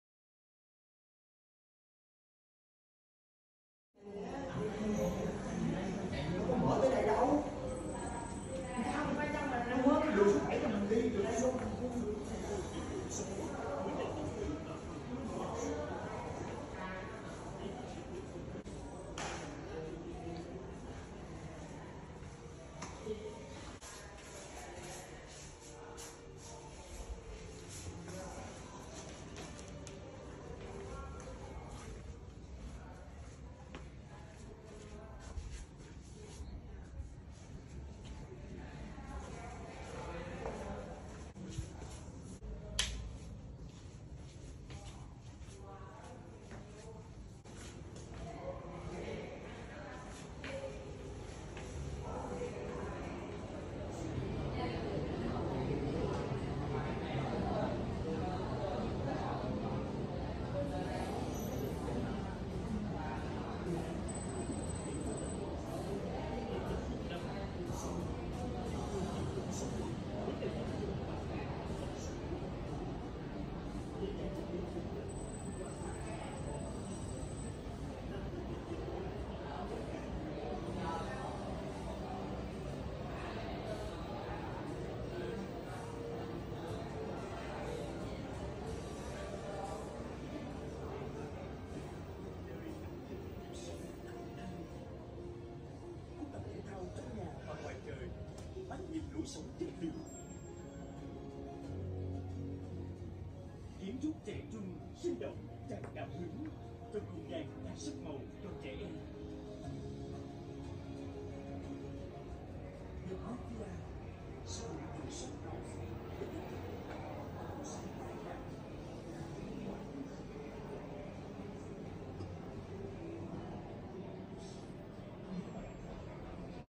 Thực tế nhà mẫu căn hộ The Privia tại Sales Gallery Khang Điền
The Privia đa dạng loại hình căn hộ: căn 1 phòng ngủ +1 diện tích từ 50-54m2, căn 2 phòng +1 diện tích từ 63-69m2 và 3 phòng ngủ diện tích 81- 83m2, hướng đến nhiều đối tượng khách hàng. Ngoài ra, khu vực "+1" sẽ được linh hoạt biến hóa công năng, phù hợp với nhu cầu và cá tính riêng của từng gia chủ.

Nội thất bên trong căn hộ đến từ các thương hiệu cao cấp như: Bộ khóa điện tử, thiết bị vệ sinh thương hiệu HAFELE, Cửa gỗ chống cháy MDF, cửa gỗ chống cháy hoàn thiện Laminate thương hiệu AN CƯỜNG , sàn gạch Porcelain, chậu rửa + vòi thương hiệu Malloca hoặc tương đương…

Dự án thiết kế thông minh, tối ưu hóa diện tích sử dụng, giàu tính duy mỹ, thống nhất từ nội thất đến cảnh quan. Những họa tiết (Pattern) cách điệu lấy ý tưởng từ hình ảnh dòng chảy được ứng dụng vào hình khối và bề mặt kiến trúc, vừa tạo sự mềm mại, vừa là điểm nhấn đặc trưng của công trình, trong tổng thể hài hòa, nhất quán, đã mang đến cho The Privia 2 giải thưởng: Best Mid End Condo Development HCMC (Dự án căn hộ Mid End tốt nhất tại TPHCM) và Best Condo Interior Design (Dự án căn hộ có thiết kế nội thất tốt nhất) tại PropertyGuru Vietnam Property Awards 2022...
Thông tin chi tiết dự án The Privia Khang Điền tại